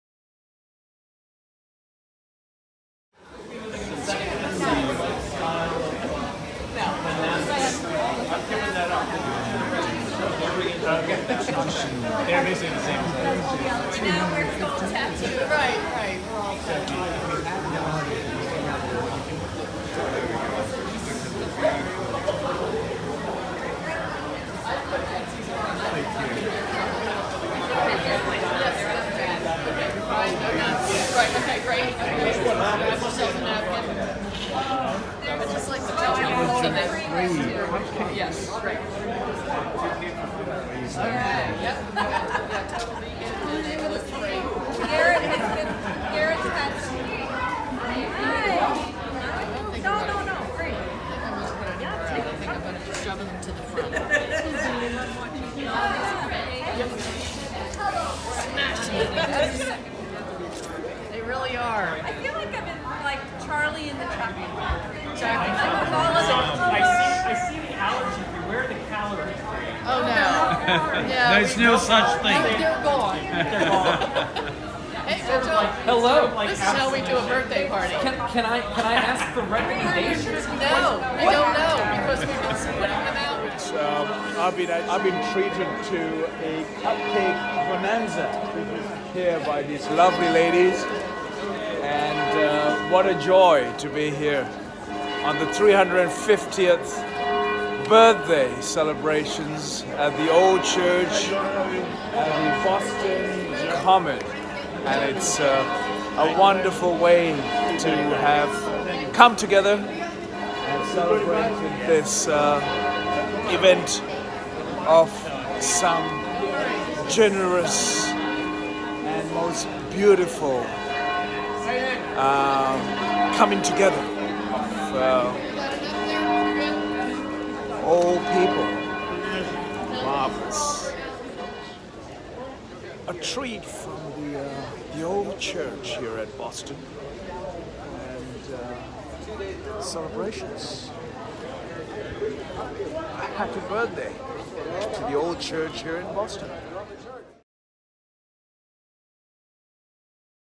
Old South Church of Boston Cupcakes Cupcakes Festival
Old South Church of Boston Cupcakes Cupcakes Festival, 350th Anniversary of the Church.